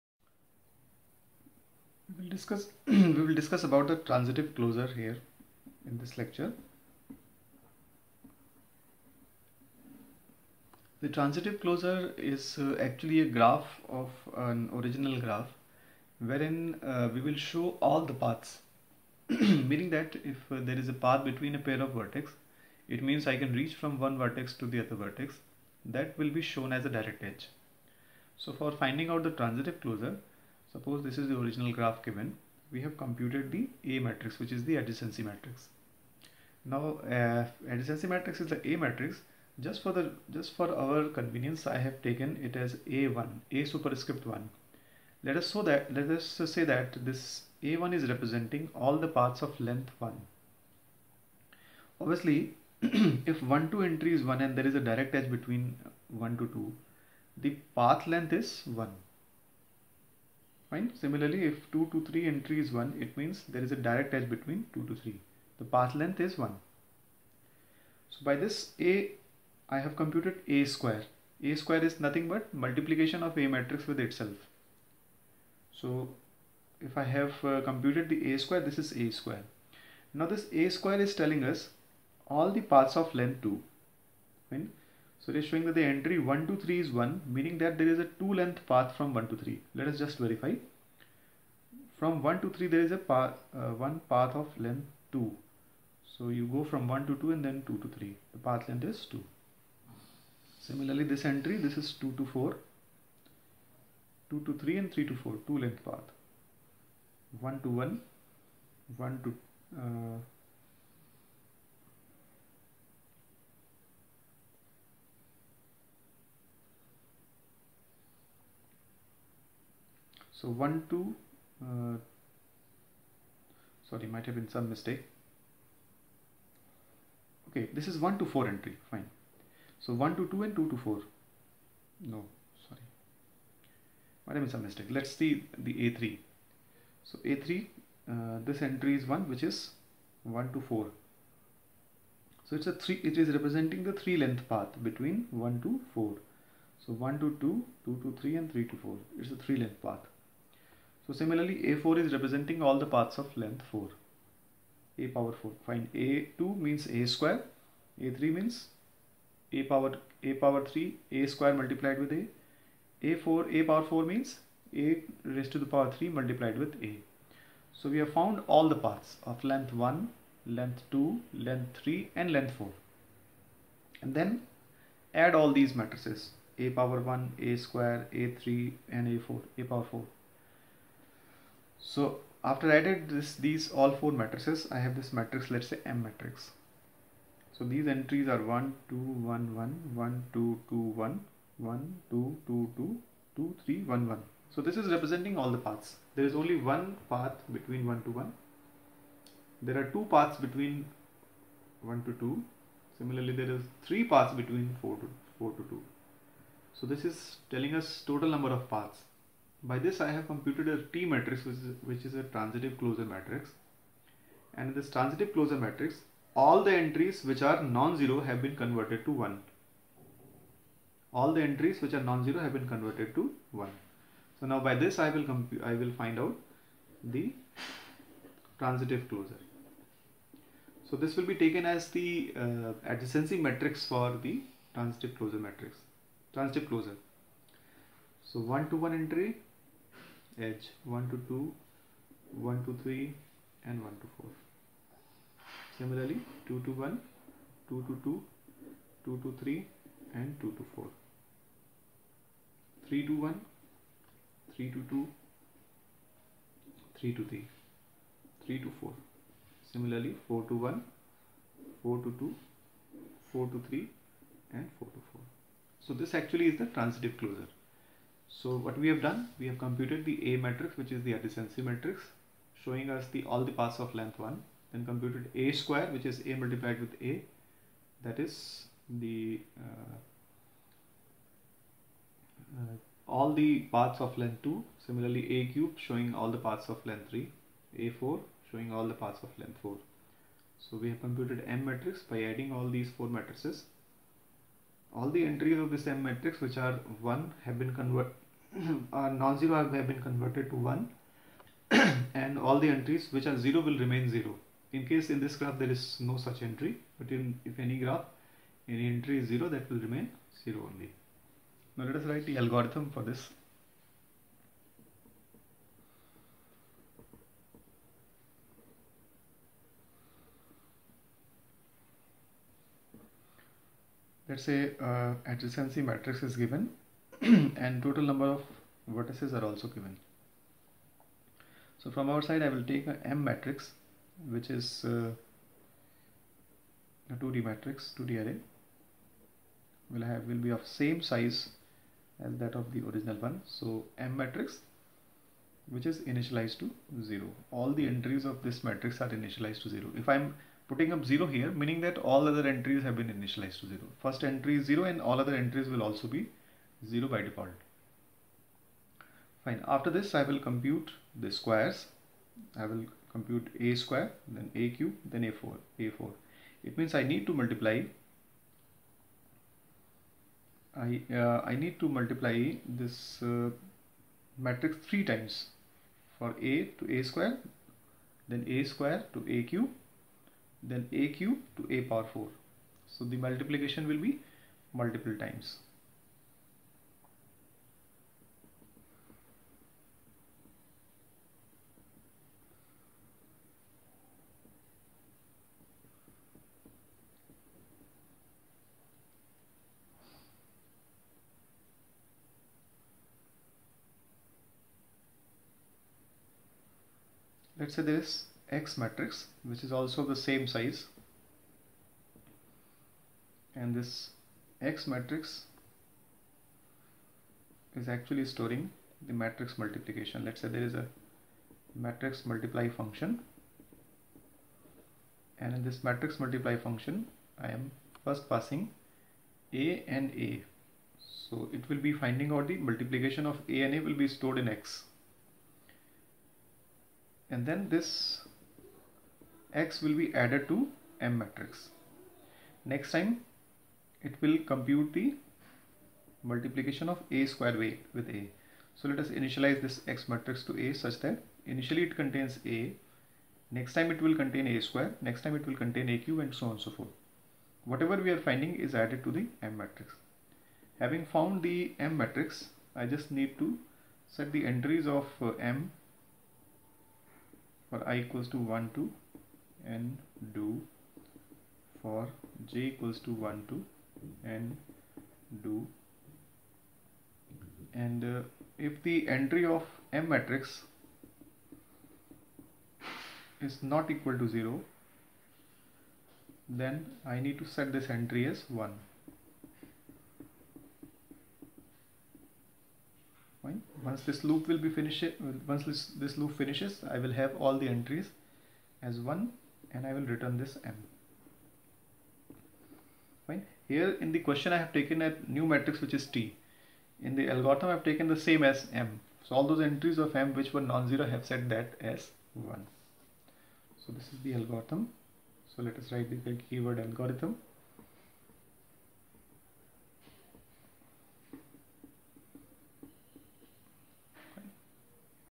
15.16 How to find Transitive Closure of a Graph
This video deals with finding the Transitive closure of the Graph using the Matrix Multiplication Method.